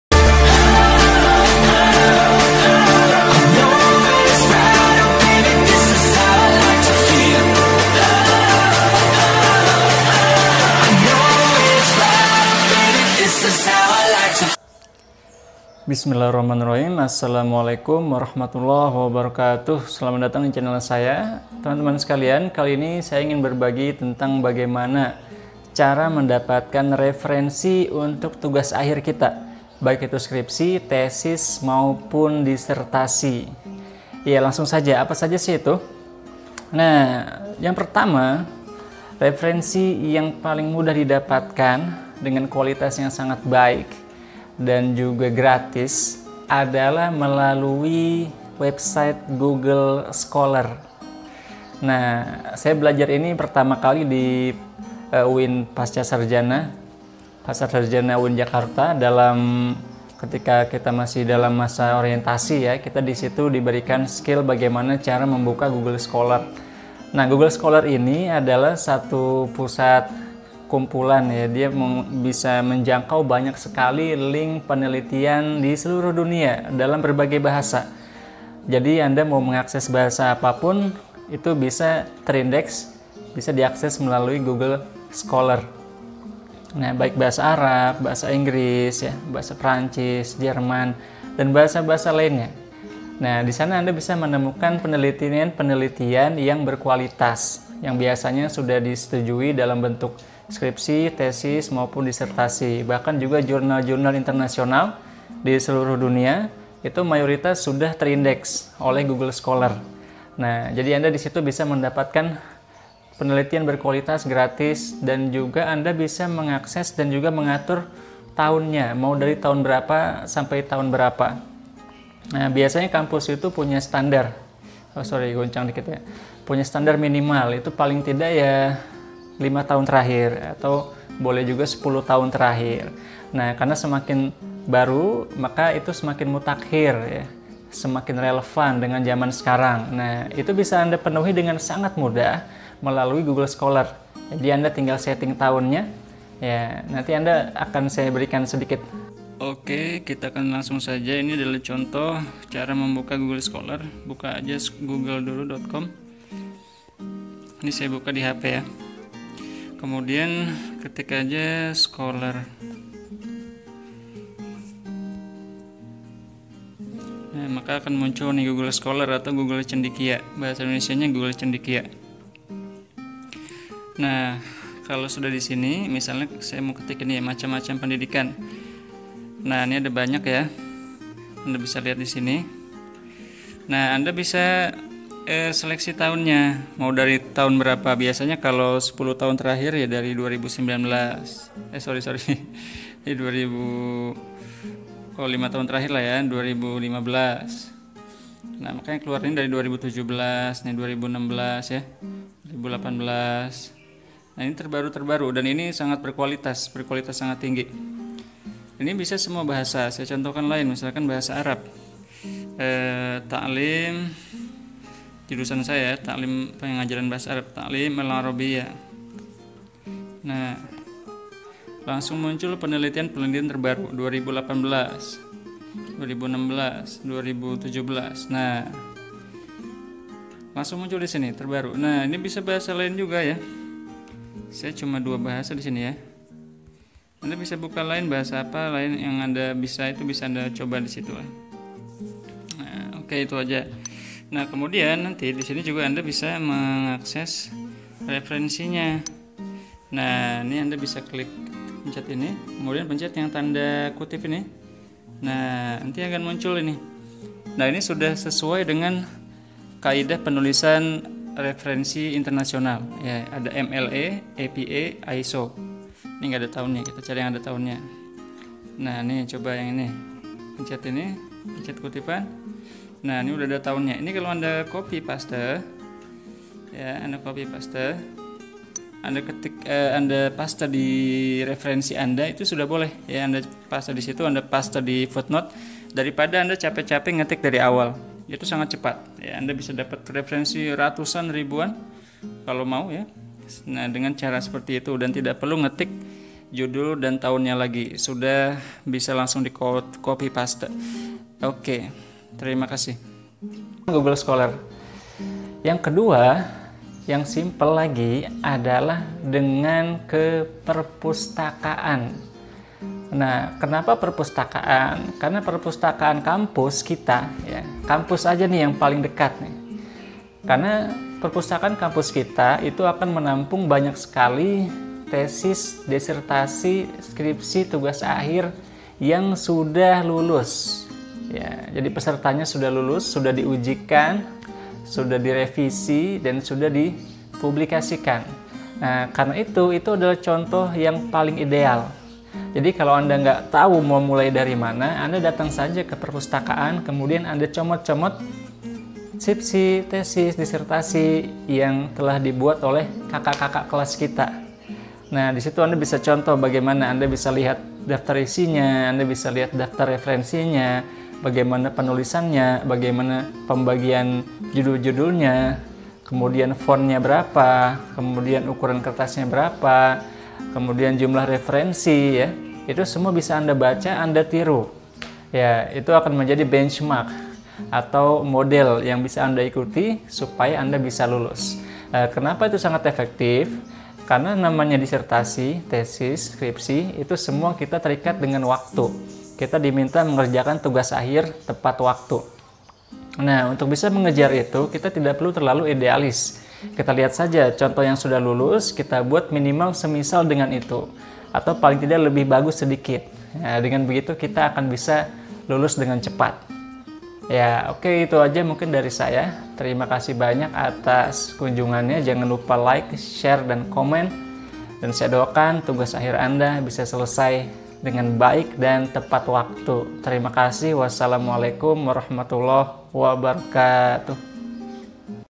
Diary Disertasi Part 37. Cara mencari referensi skripsi, tesis, dan Disertasi
dalam video ini saya menjelaskan dua cara paling efektif untuk mencari referensi tugas akhir baik itu skripsi, tesis, disertasi, maupun artikel jurnal, karya tulis dan penelitian.

tonton sampai selesai ya, mudah2an bermanfaat

terima kasih